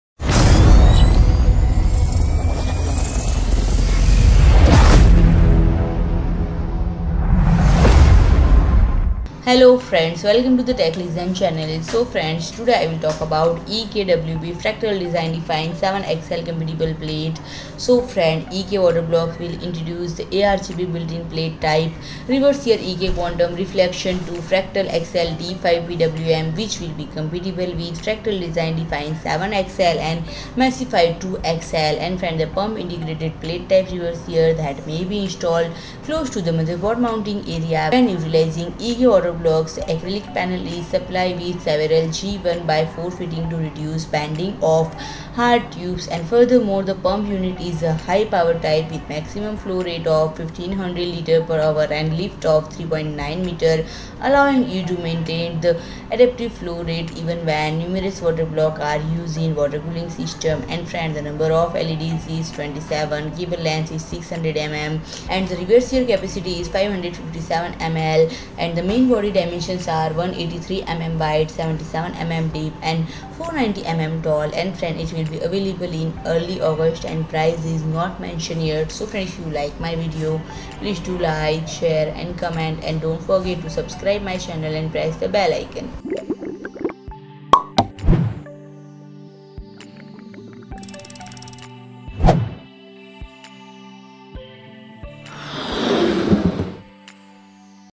EK-Quantum Reflection² Fractal XL D5 PWM D-RGB Launched - Price Form 409 EUR - All Spec, Features
PRODUCT DETAILS & SIMILAR PRODUCT ORDER FROM AMAZON

EK Water Blocks will introduce the ARGB built-in plate type reservoir EK-Quantum Reflection2 Fractal XL D5 PWM, which will be compatible with Fractal Design Define 7 XL and Meshify 2 XL. A pump-integrated plate-type reservoir that may be installed close to the motherboard mounting area. When utilizing EK Water Blocks water blocks, the acrylic panel is supplied with several G1 / 4 fittings to reduce bending of hard tubes. Furthermore, the pump unit is a high-power type with a maximum flow rate of 1,500 L / h and a lift of 3.9 m, allowing it to maintain an adequate flow rate even when numerous water blocks are used in the water cooling system. The number of LEDs is 27, the cable length is 600 mm, the reservoir capacity is 557 ml, and the main body dimensions are 183 mm wide, 77 mm deep, and 490 mm tall.